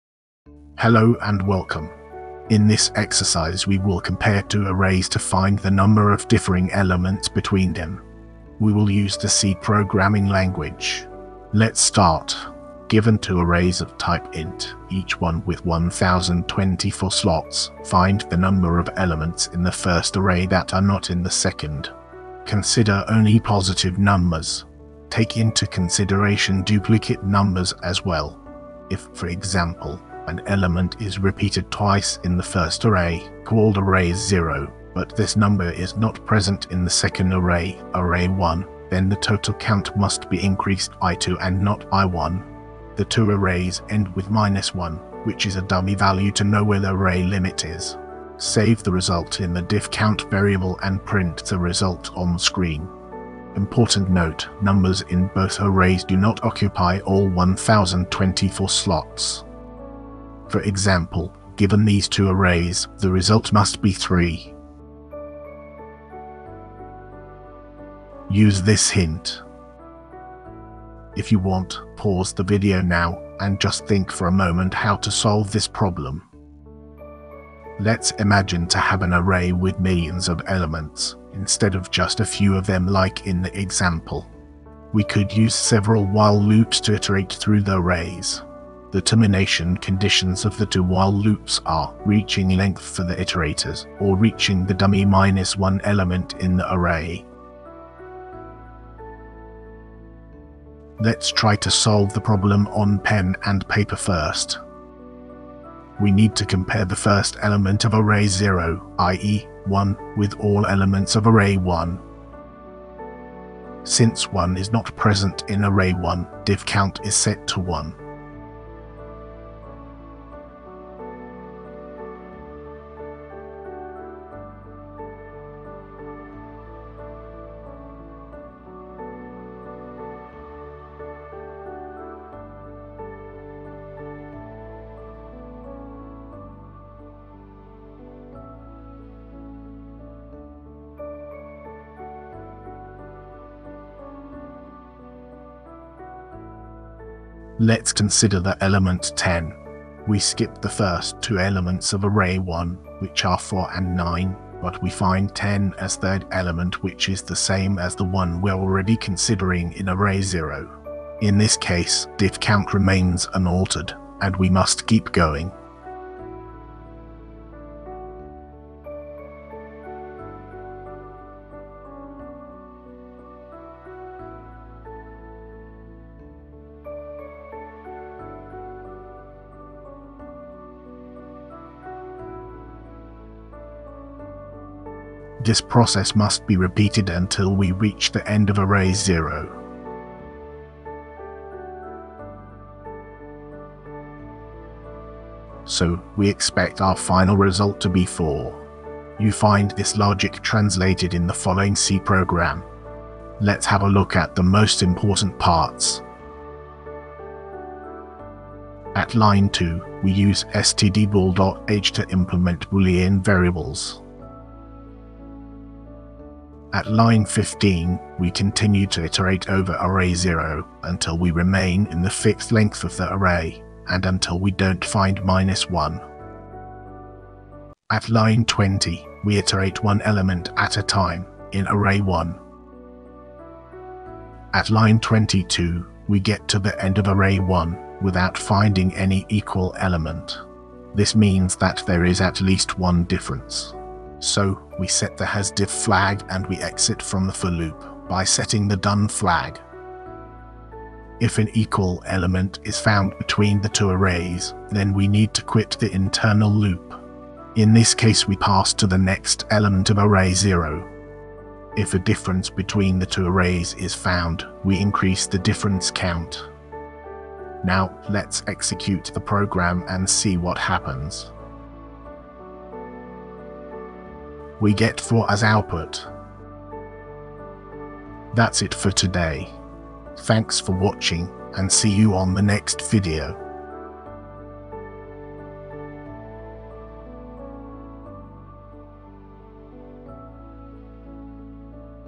Find differing elements between two arrays in C
In this video I guide you through an exercise about finding differing elements between two arrays. The implemented algorithm finds the number of elements of the first array that are not in the second.

The presented solution is written in the C programming language.